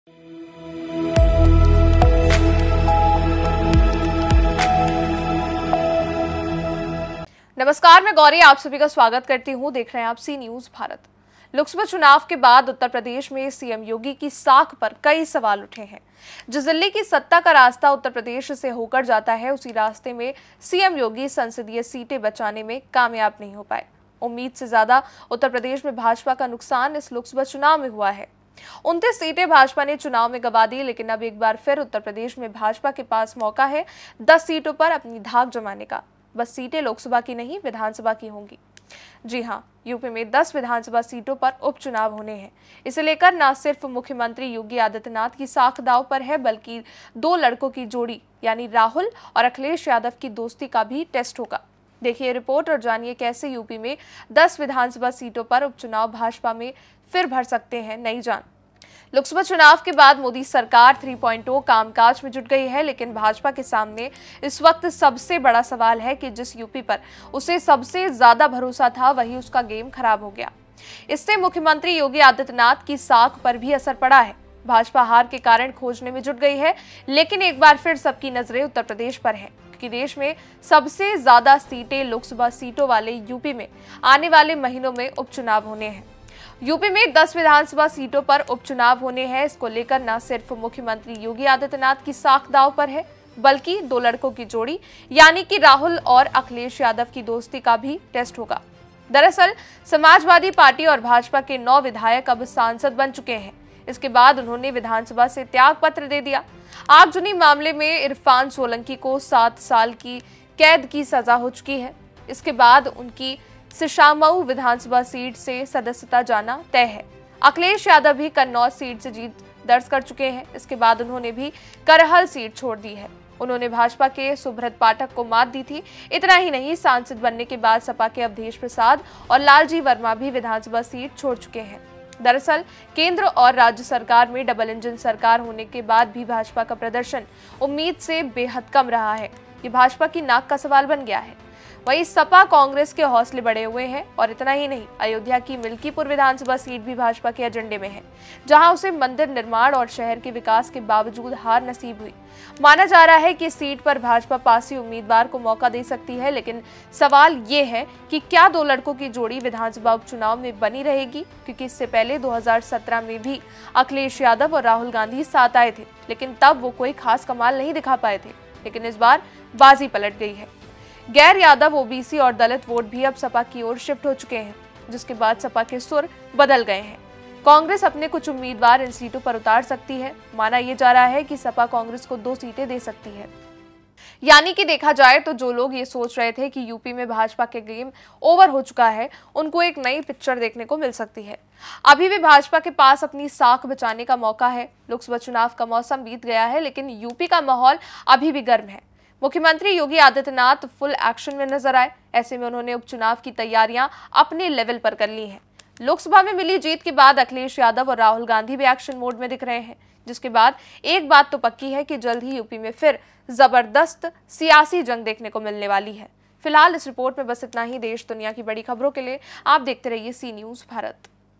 यूपी में 10 विधानसभा सीटों पर उपचुनाव, इस बार बाजी किसकी ? || Cnews Bharat
चुनाव नतीजों के बाद एक्शन में CM योगी

सीएम योगी की सांख पर कई सवाल उठे

यूपी में 10 विधानसभा सीटों पर उपचुनाव होने हैं

अखिलेश यादव और राहुल गांधी भी एक्शन में दिख रहे